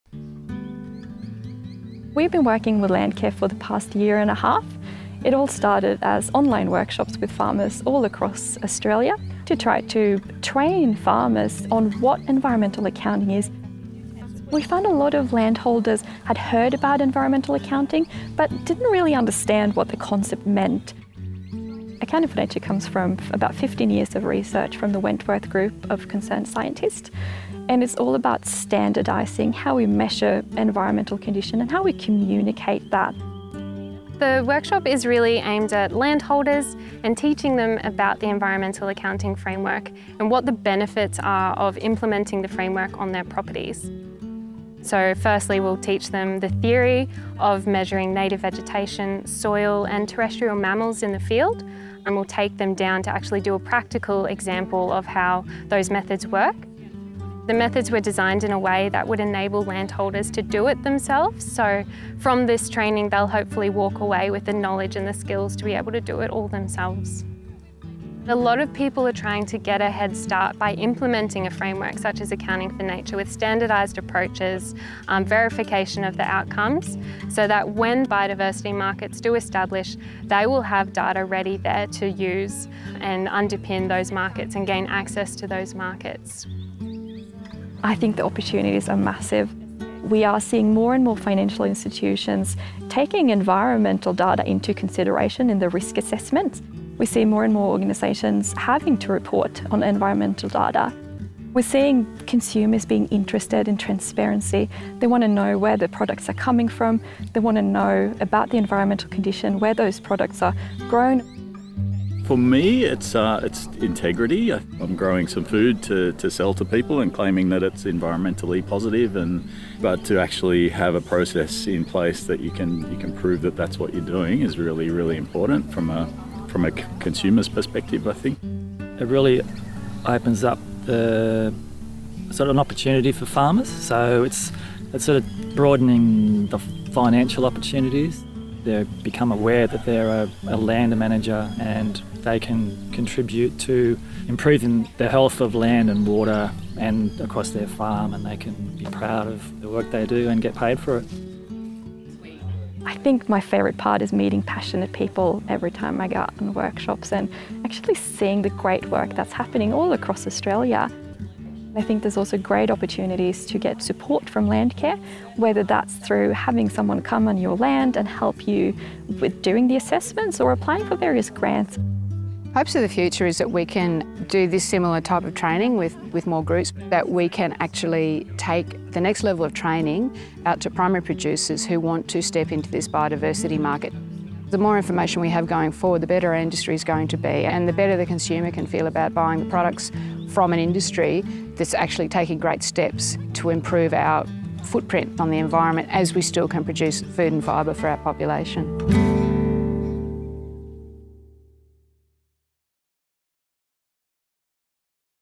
Landcare Farming - Natural Capital Accounting Benchmarking Project
The Natural Capital Accounting Benchmarking project (NCA), is a joint project between Accounting for Nature® Ltd and Landcare Farming to provide training in natural capital accounting methods and develop environmental baseline reports for producers.